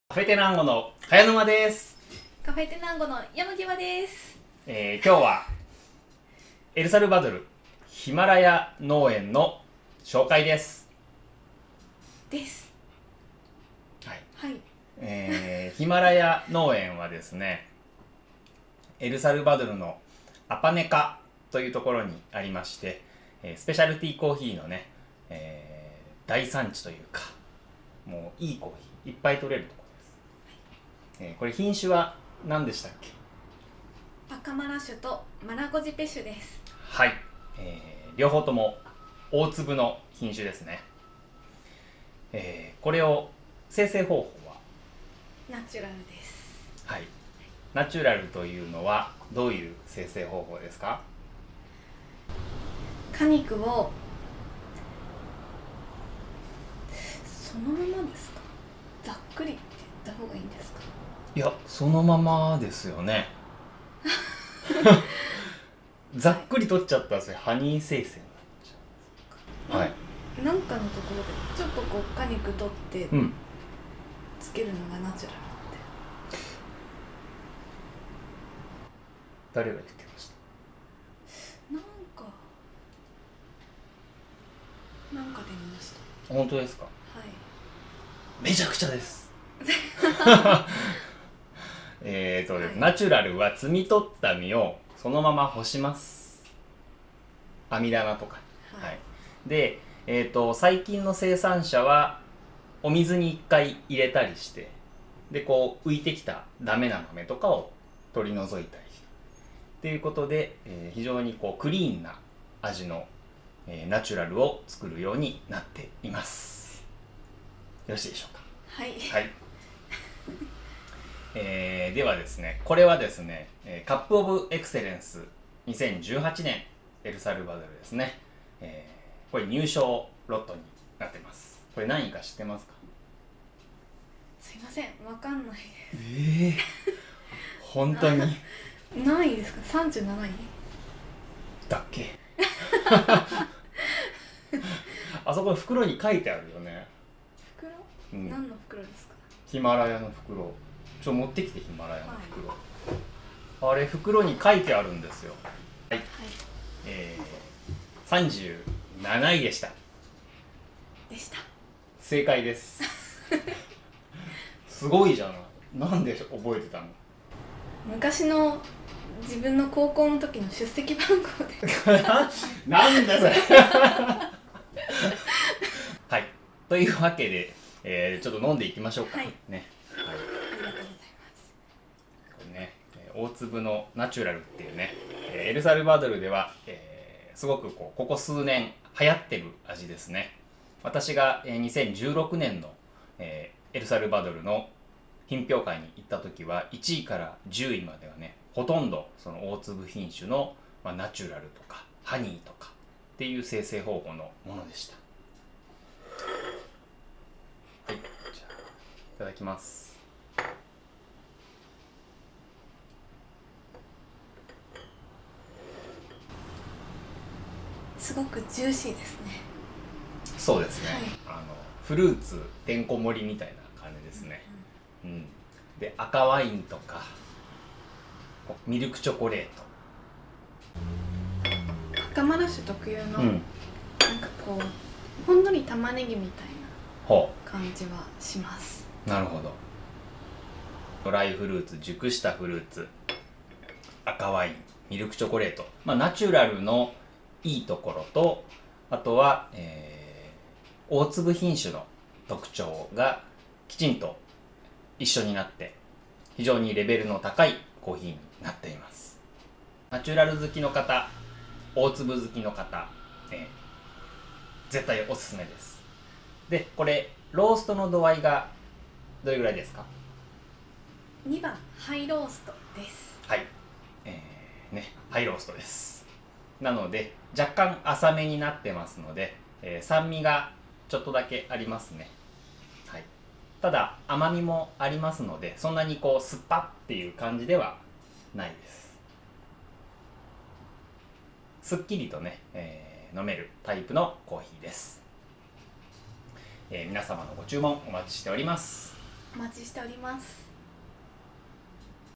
ヒマラヤ農園紹介動画
カフェテナンゴが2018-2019年に販売しているエルサルバドルのヒマラヤ農園を紹介する動画です。パカマラとマラゴジペをナチュラル精製した2018年COE＃37のロットです。